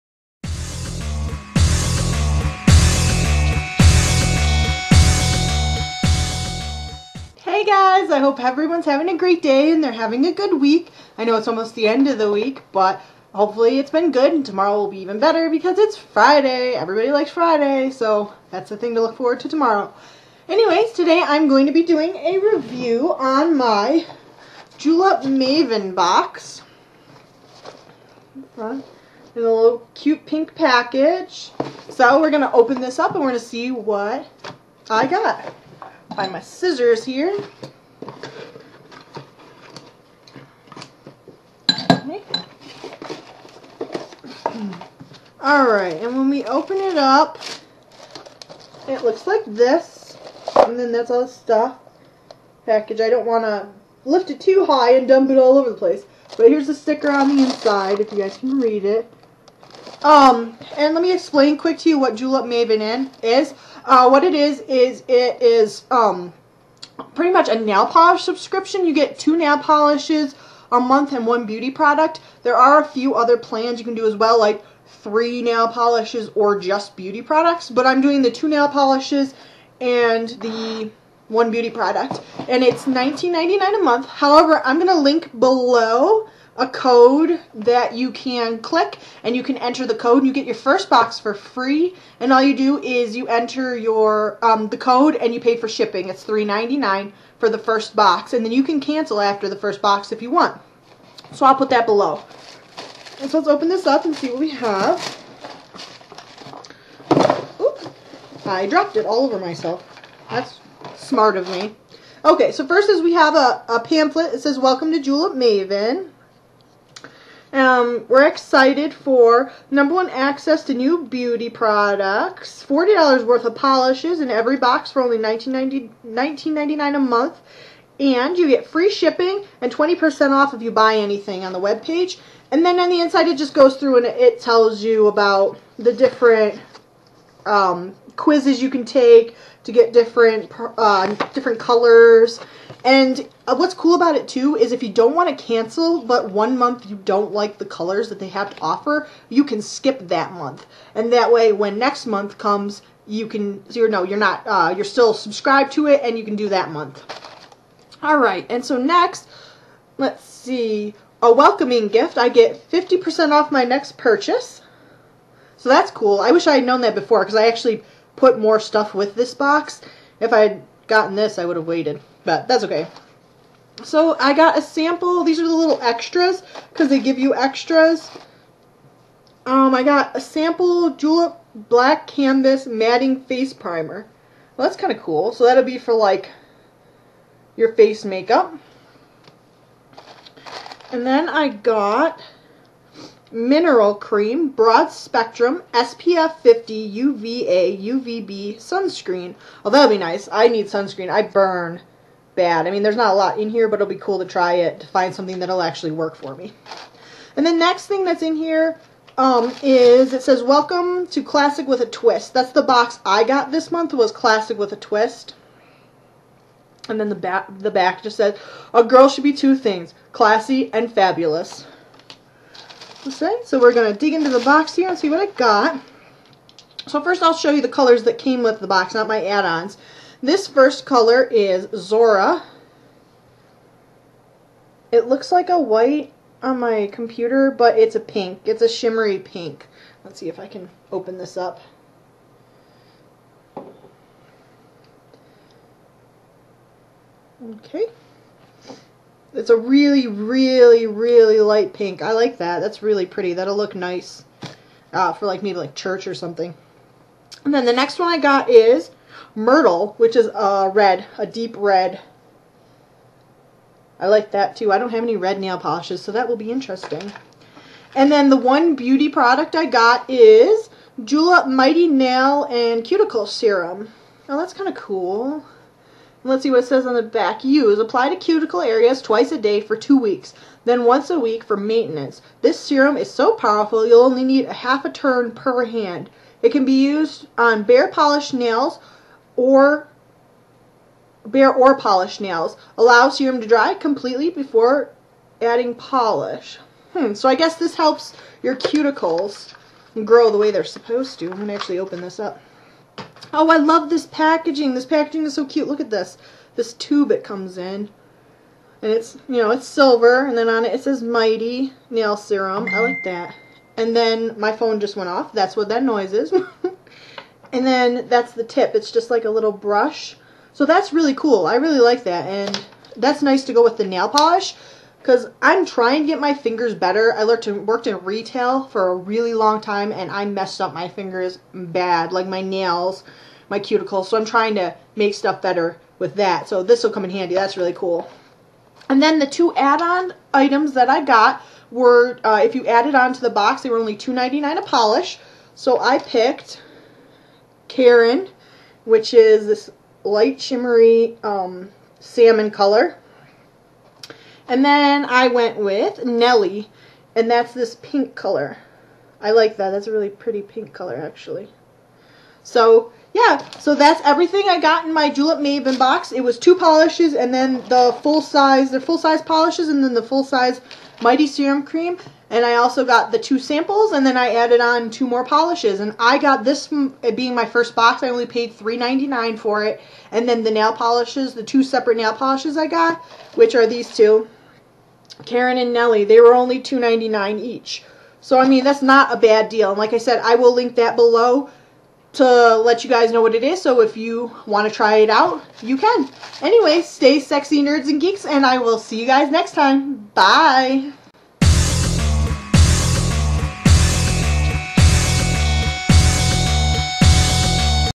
Julep Mavin Unboxing January
This is my unboxing for Julep Mavin. Click on my link before and get your first box free, only pay 3.99 for shipping. You get 2 nail polishes a month, and 1 beauty item a month. Along with 1 or 2 free samples.

Julep Mavin (when at the check out use the code FREEBOX)